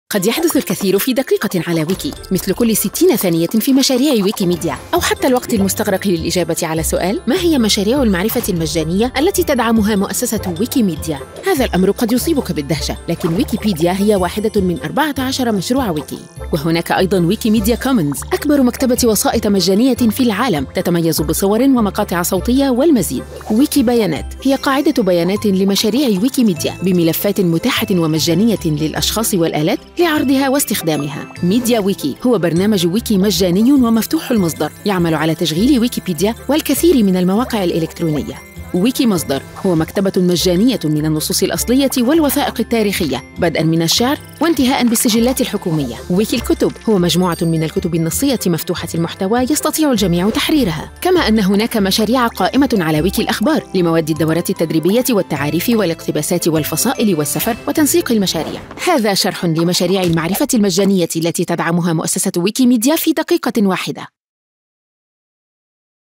ما هي مشاريع المعرفة المجانية التي تدعمها مؤسسة ويكيميديا؟ | A WIKI MINUTE
هل كنتم تعلمون بأن ويكيبيديا هو واحد من 14 مشروع معرفي حر تدعمها مؤسسة ويكيميديا؟ هناك كون من المشاريع لإتاحة المعرفة أمام الجميع وفي كل مكان. شاهدوا لتتعلموا المزيد.

مؤسسة ويكيميديا، المنظمة غير الربحية وراء ويكيبيديا والمشاريع المعرفية المجانية الأخرى، صنعت "دقيقة على ويكي" وهي عبارة عن سلسلة فيديوهات لتجيب عن الأسئلة المتكررة المتعلقة بمشاريع ويكيميديا في دقيقة واحدة.